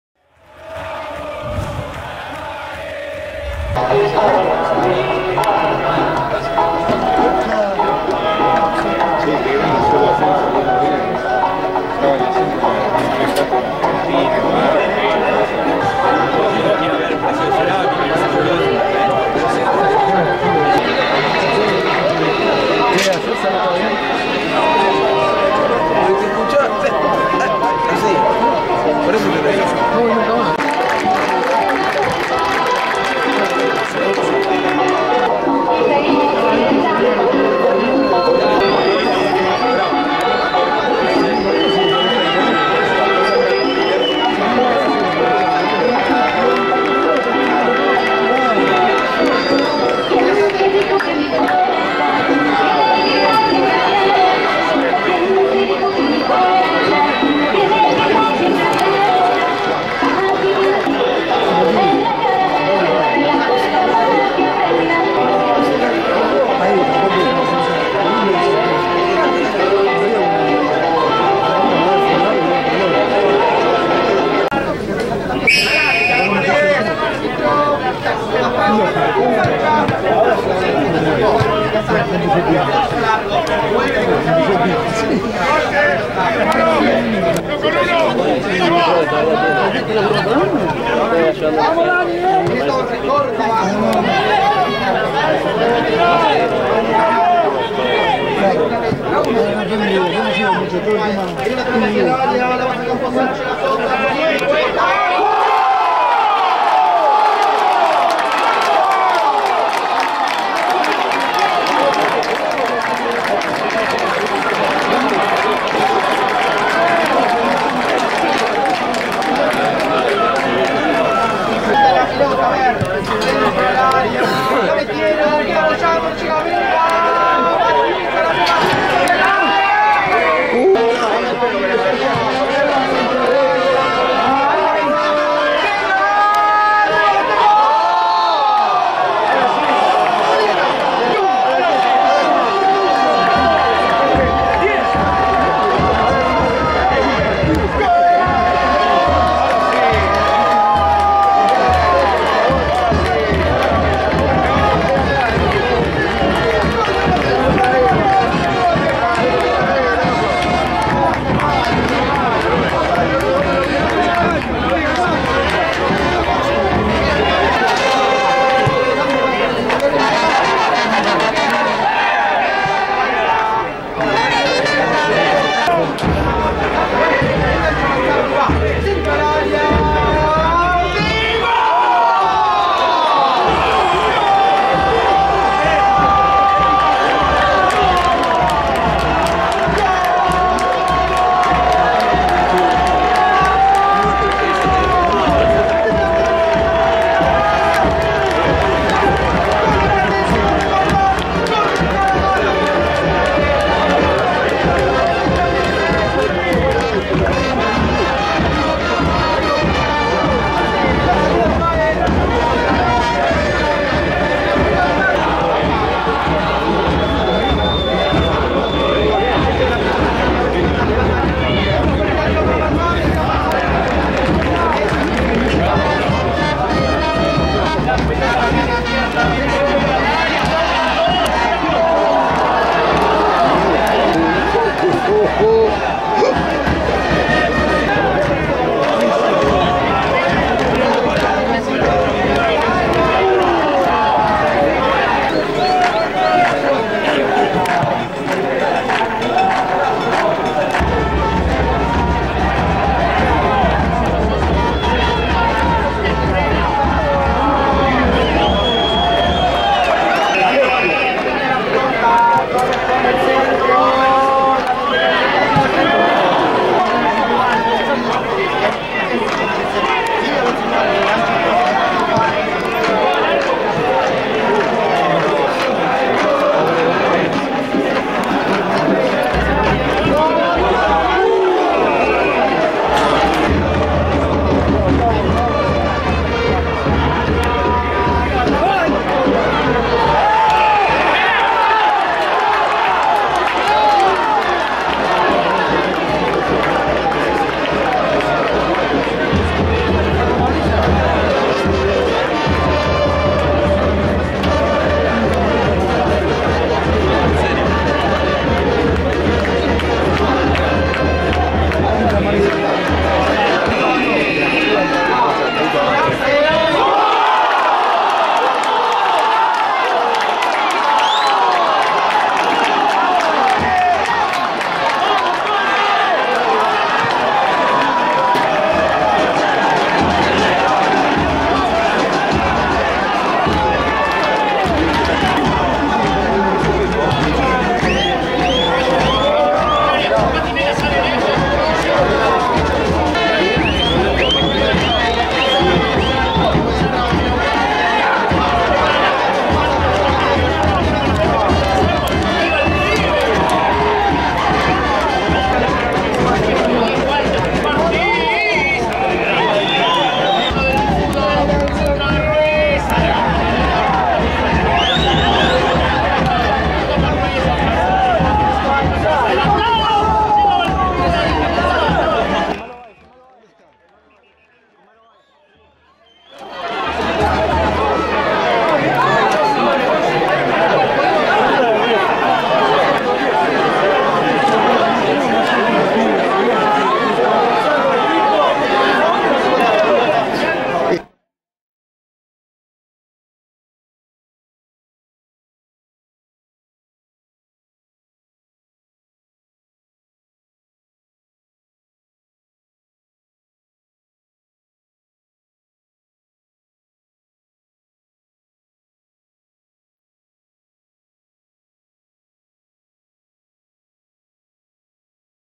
Colegiales 2 - 2 Platense | Fecha 39 | Campeonato 2013/2014
Video con los goles y las mejores jugadas de la igualdad de Platense por 2-2 ante Colegiales, por el partido correspondiente a la 39° jornada del Campeonato 2013/2014 de la Primera "B" Metropolitana, en el estadio "Libertarios Unidos". Arbitraje de Hernán Mastrángelo. Goles de Molina y Ortiz para el Calamar.